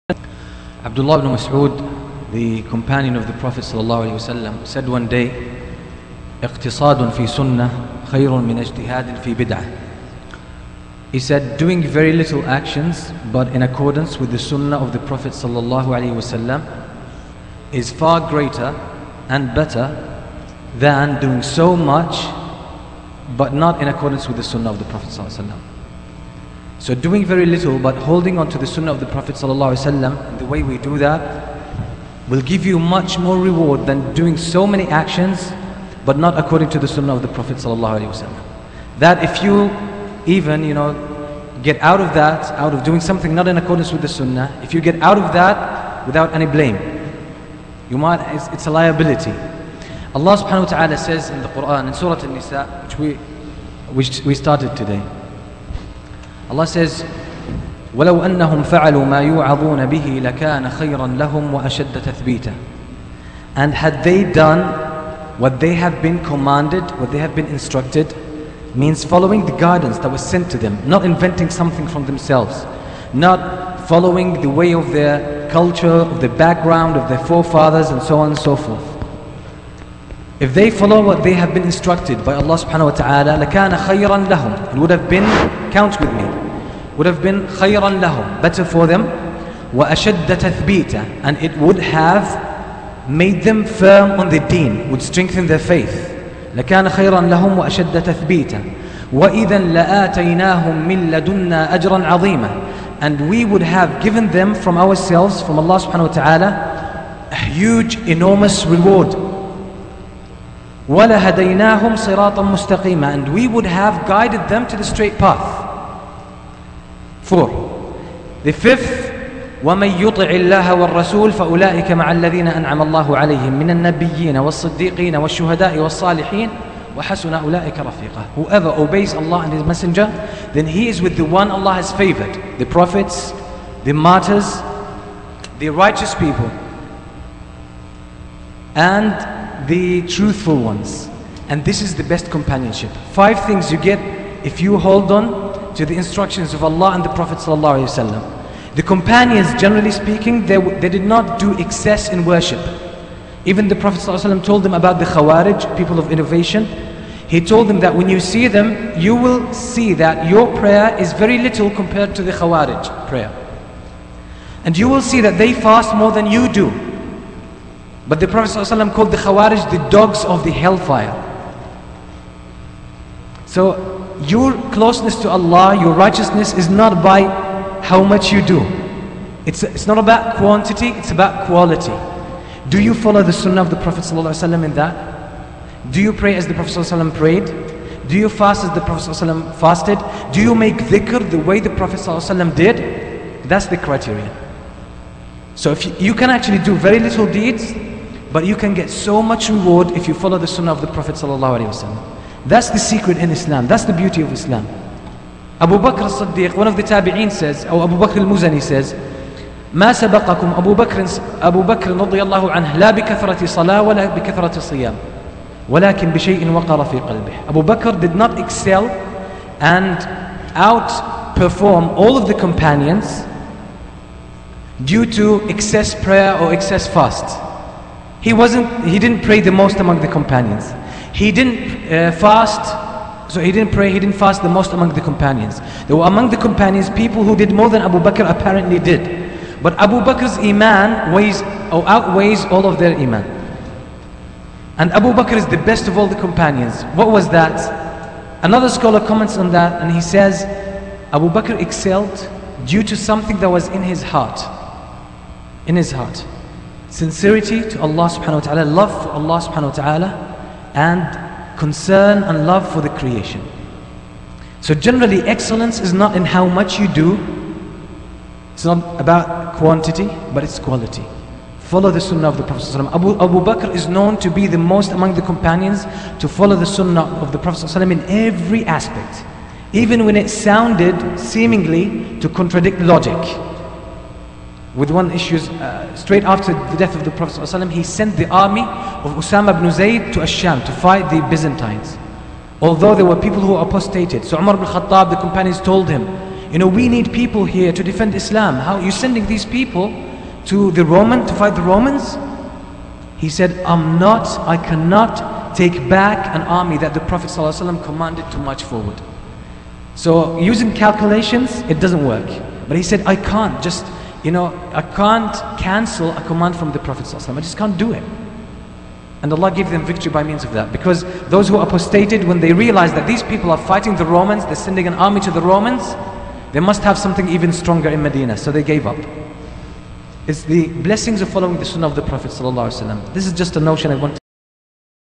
Quality over Quntity - Mutassim al Hamedi
It is undoubly preferred to do few things in good quality than do many things in poor quality

And one of the major barometers of quality is how much the action you do adheres to the Qur'an and the Authentic Sunnah.